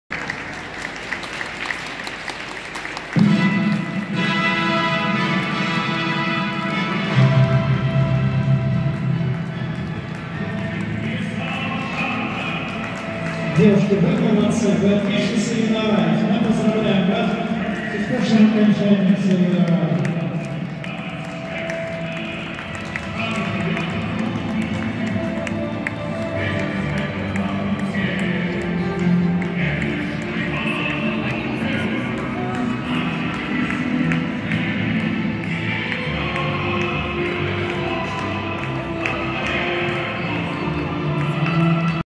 Завершение 1-го дня: многоборки - 05.12.2020 (18:54:02)
5-6 декабря 2020 года в Легкоатлетическом манеже ГБУ СШОР «Академия лёгкой атлетики Санкт-Петербурга» прошли традиционные соревнования по лёгкой атлетике «Открытие зимнего спортивного сезона» среди юношей и девушек до 16 лет.